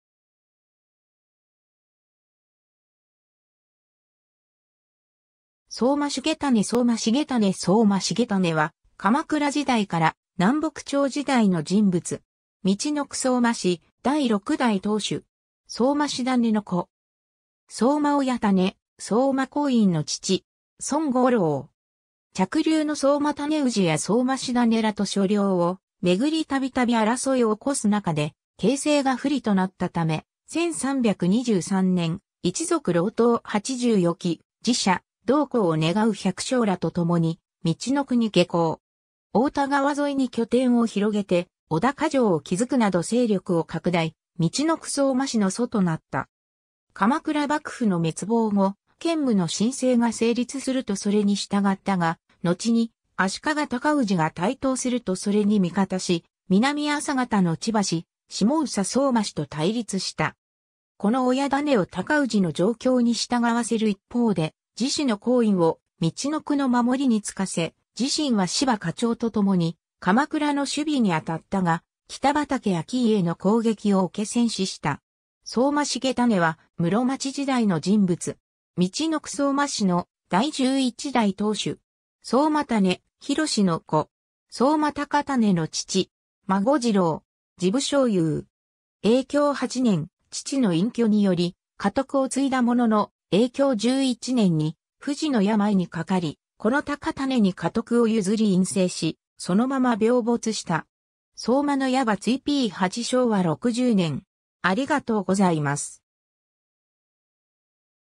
相馬重胤
相馬重胤（そうま　しげたね） 相馬重胤 相馬 重胤（そうま しげたね、 生年不詳 - 延元2年/建武4年（1337年））は、鎌倉時代から南北朝時代の人物。
陸奥相馬氏第6代当主。
相馬師胤の子。
相馬親胤、相馬光胤の父。
孫五郎。
 嫡流（下総相馬氏）の相馬胤氏や相馬師胤（重胤の父とは別人）らと所領を巡りたびたび争いを起こす中で、形勢が不利となったため、1323年、一族郎党80余騎、寺社、同行を願う百姓らとともに陸奥に下向。
太田川沿いに拠点を広げて小高城を築くなど勢力を拡大、陸奥相馬氏の祖となった。
鎌倉幕府の滅亡後、建武の新政が成立するとそれに従ったが、のちに足利尊氏が台頭するとそれに味方し、南朝方の千葉氏、下総相馬氏と対立した。
子の親胤を尊氏の上京に従わせる一方で、次子の光胤を陸奥の守りにつかせ、自身は斯波家長と共に鎌倉の守備にあたったが、北畠顕家の攻撃を受け戦死した。
 相馬 重胤（そうま しげたね）は、室町時代の人物。
陸奥相馬氏の第11代当主。
相馬胤弘の子。
相馬高胤の父。
孫次郎。
治部少輔。
 永享8年（1437年）、父の隠居により家督を継いだものの、永享11年（1440年）に不治の病にかかり、子の隆胤（のちの高胤）に家督を譲り隠棲し、そのまま病没した。
 ^ [相馬の野馬追p8 （相馬野馬追保存会）昭和60年]